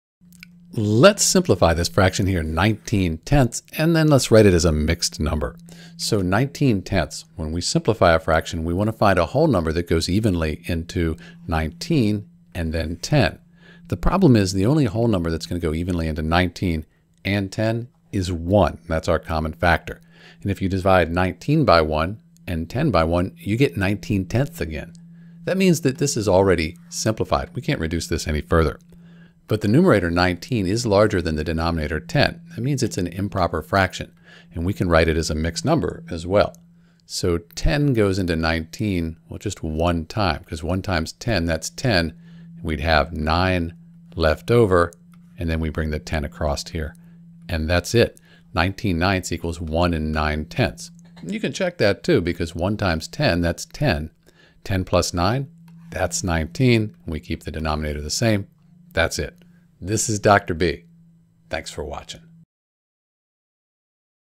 How to Simplify the Fraction 19/10 (and as a Mixed Fraction)
If we divide 19 by 1, we get 19. If we divide 10 by 1, we get 10.

Now, if we want to convert 19/10 to a mixed number, here's how we do it:

To get 19/10 as a mixed number, we divide the numerator (19) by the denominator (10). The result is 1 with a remainder of 9. This means that 19/10 can be written as the mixed number 1 9/10. The whole number 1 represents how many times 10 goes into 19, and the fraction 9/10 represents the remainder.